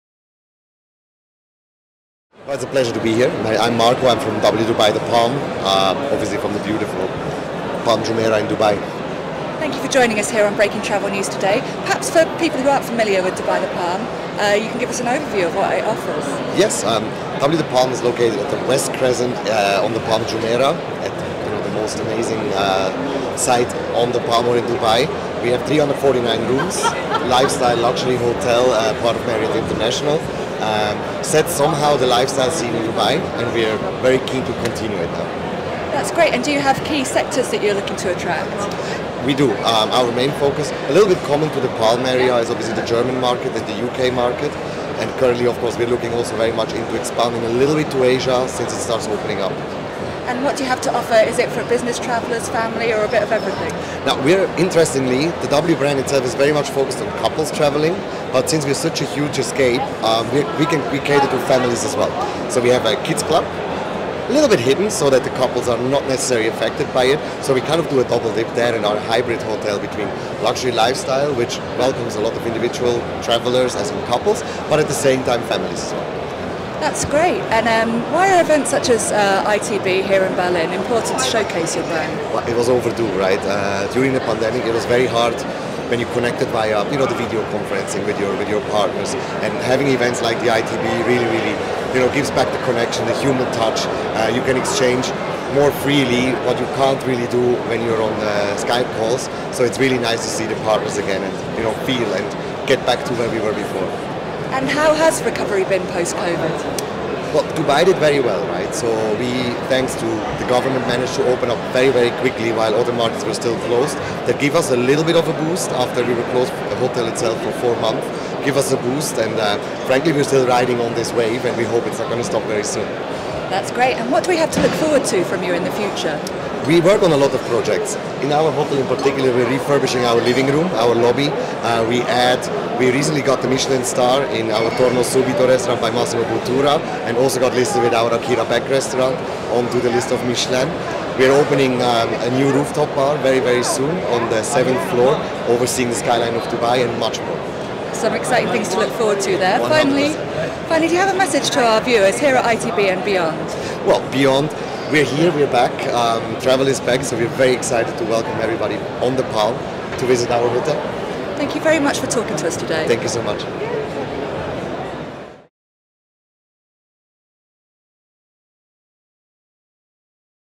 ITB Berlin 2023: Marco M. Neukom, Director of Operations, W Dubai - The Palm
talks to Breaking Travel News at ITB Berlin 2023.